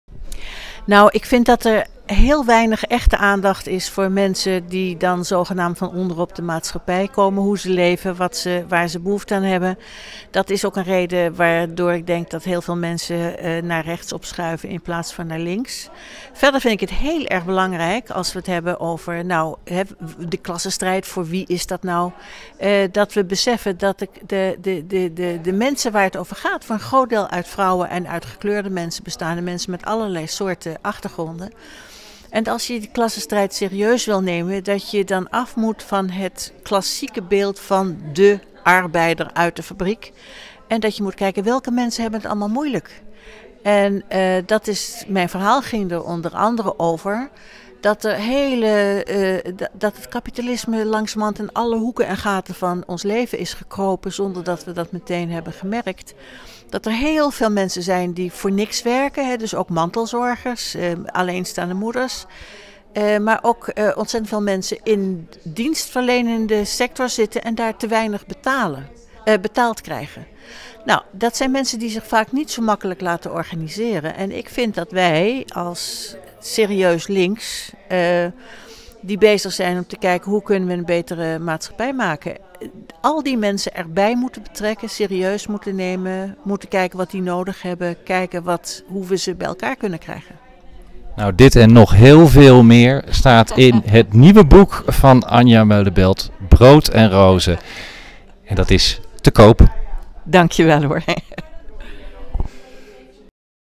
Interview met Anja Meulenbelt
Kort interview met socialist en feminist Anja Meulenbelt over haar nieuwe boek 'brood en rozen - over identiteit en klasse'. Tijdens Marxisme Festival, georganiseerd door de Internationale Socialisten.